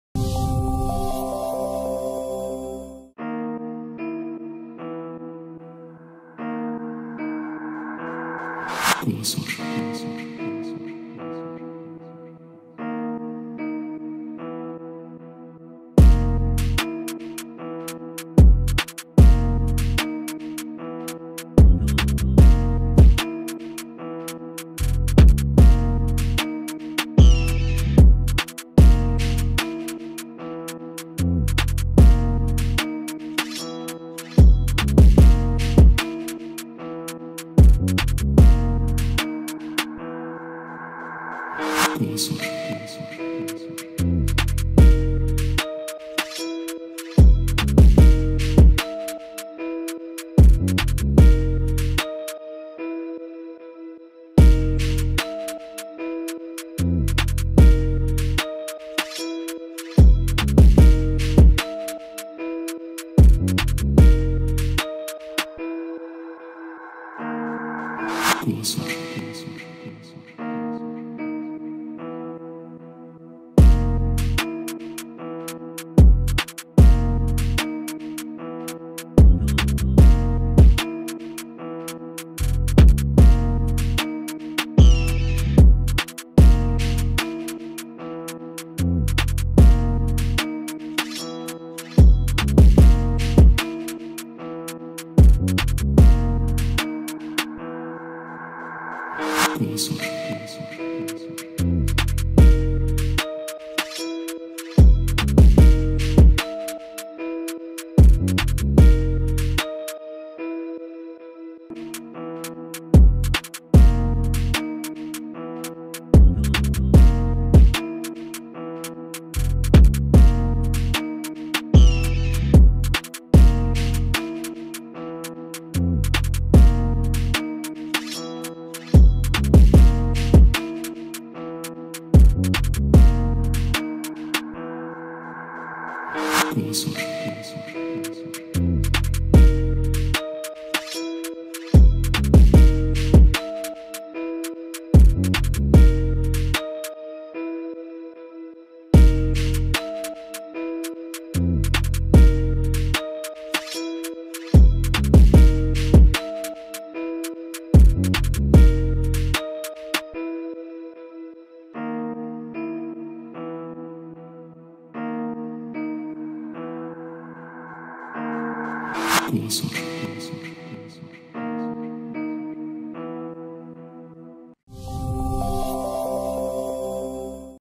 UKRAINE WAR/Drone targets Russians who were carrying wounded by boat they tried to escape by jump..
UKRAINE WAR/Drone targets Russians who were carrying wounded by boat  they tried to escape by jumping into water